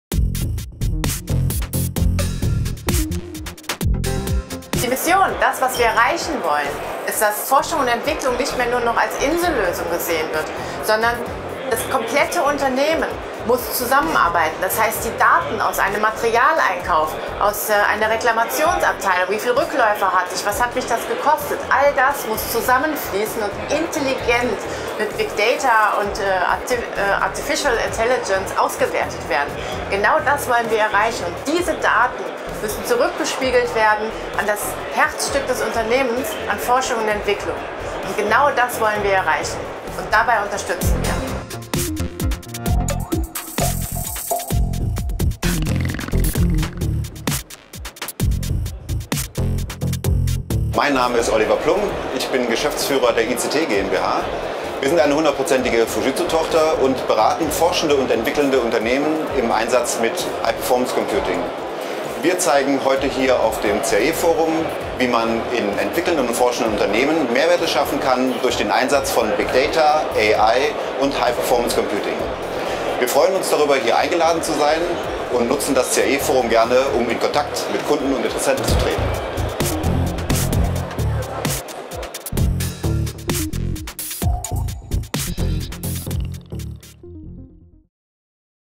CAE-Forum | Statements Petra Pfeffer und Oliver Plum
Interviews mit Frau Petra Pfeffer und Herrn Oliver Plum während der Hannover Messe vom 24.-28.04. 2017